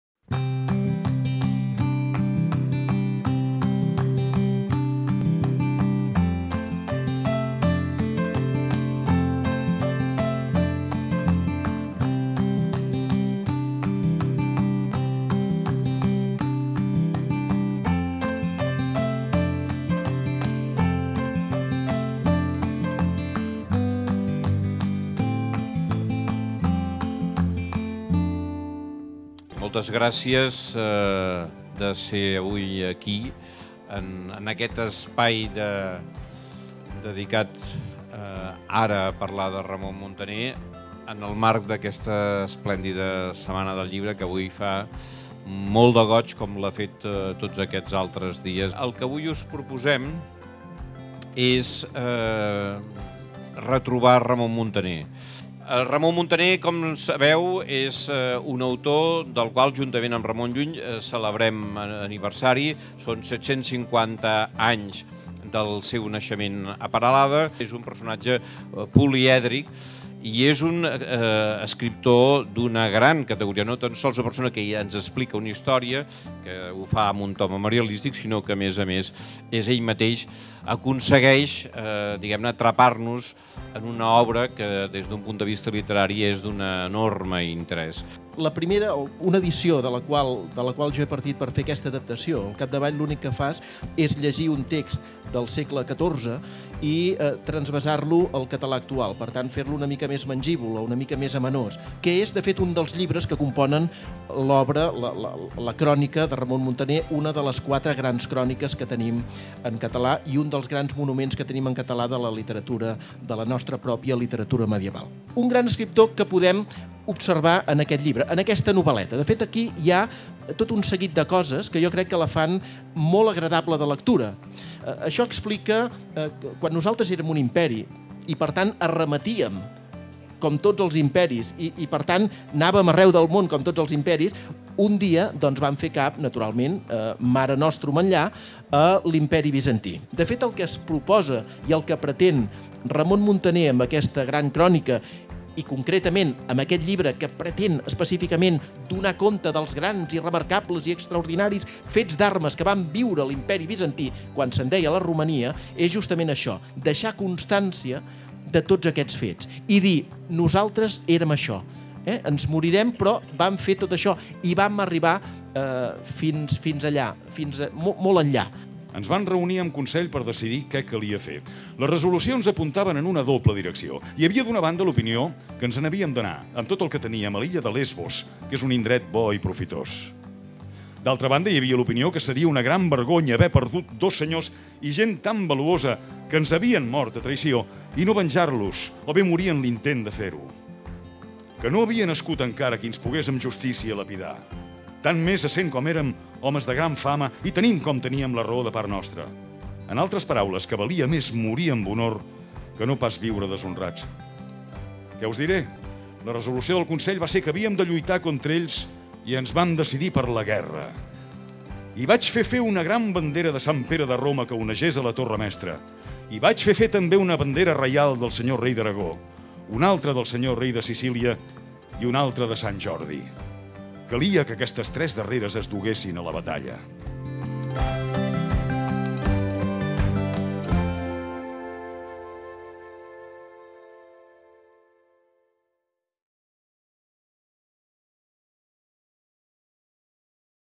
Lectura de “L’expedició dels catalans a orient” de Ramon Muntaner
Resum de l'acte de presentació de 'Lexpedició dels catalans a Orient' de Ramon Muntaner. En l'acte vam poder escoltar Carles Duarts, Director de la Fundació Carulla, Jordi Llavina, responsable de la versió al català modern del llibre, i l'actor Jordi Boixaderes.